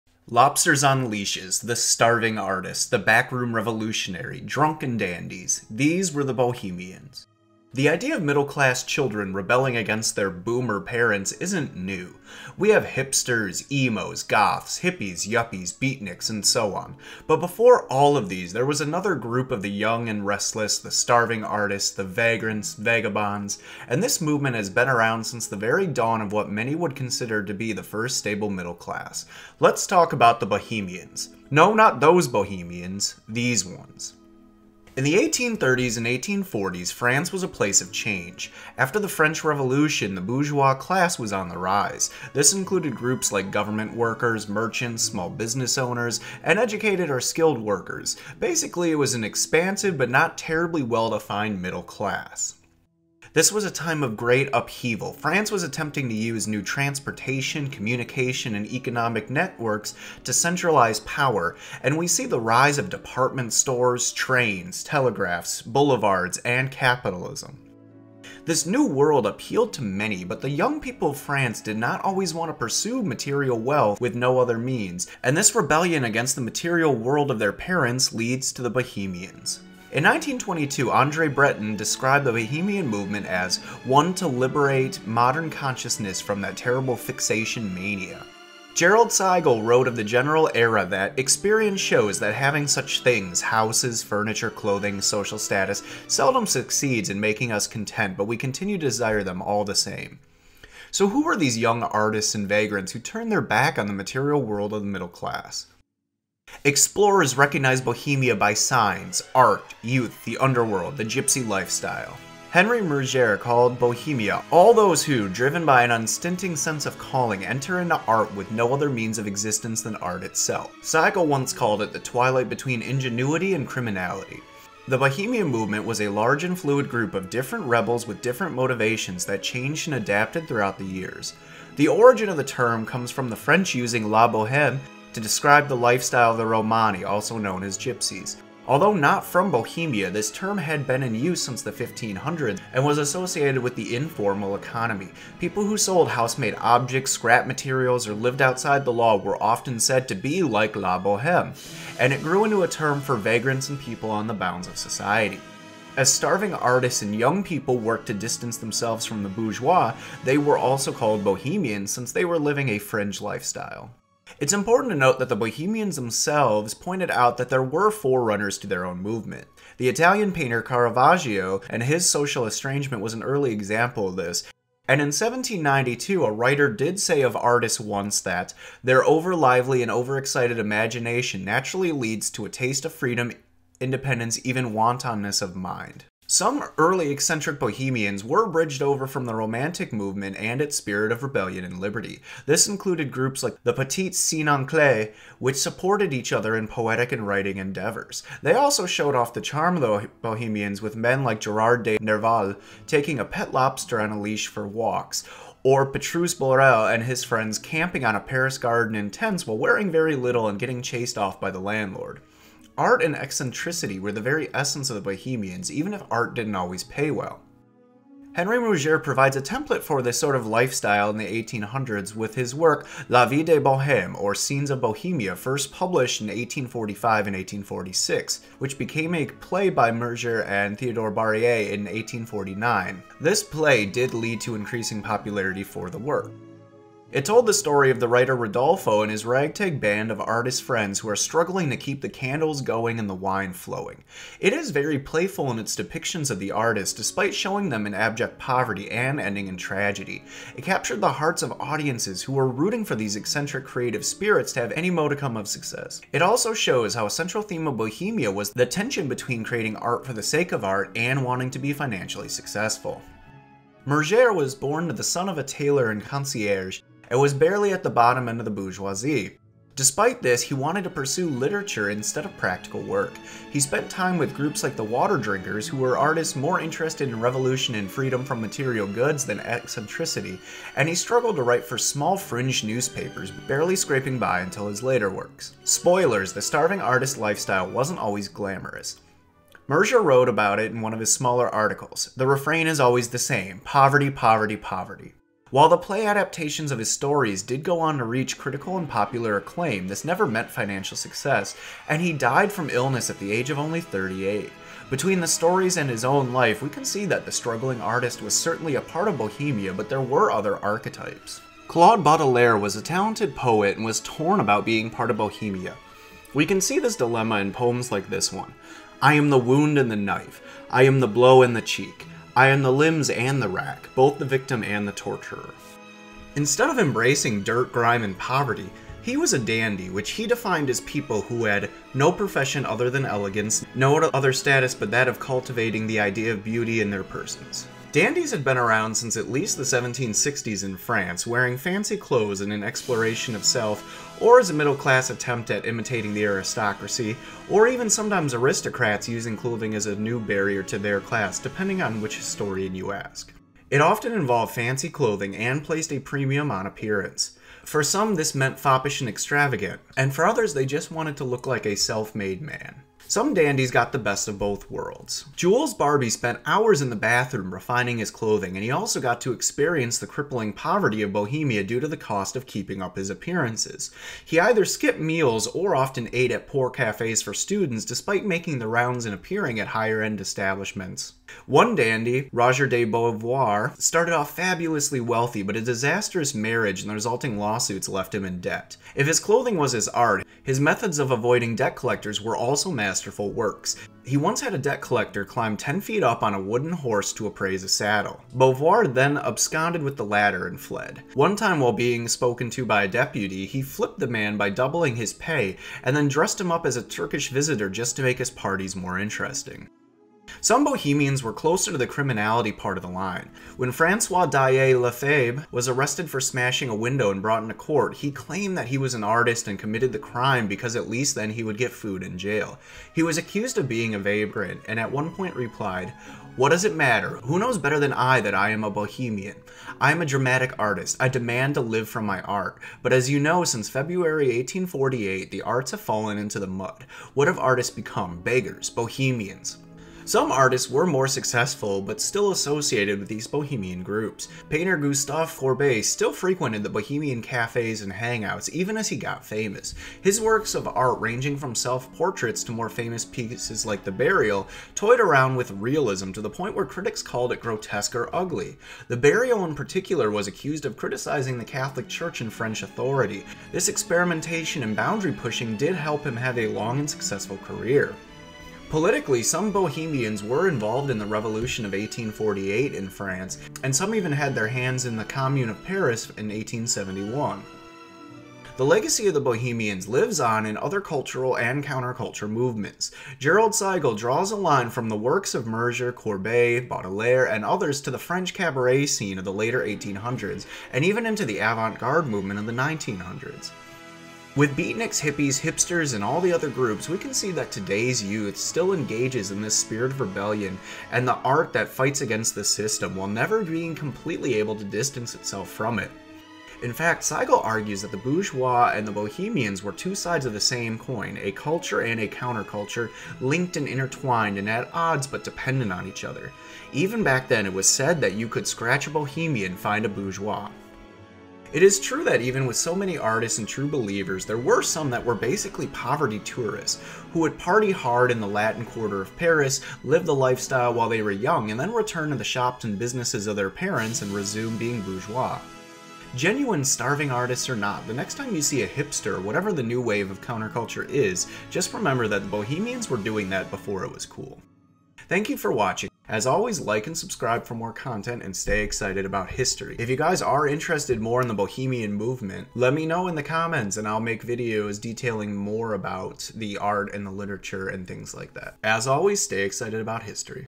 Bohemians in Paris - Hipsters Before It Was Cool
The Bohemians in Paris were an early counter-culture movement that made art and stood against the middle class pursuit of wealth before it was cool.  Come watch these early hipsters as we explores, dandies, starving artists, vagrants, revolutionaries, poets, and more.

Much of this comes from Jerrold Seigel's "Bohemian Paris: Culture, Politics and the Boundaries of Bourgeois Life, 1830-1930"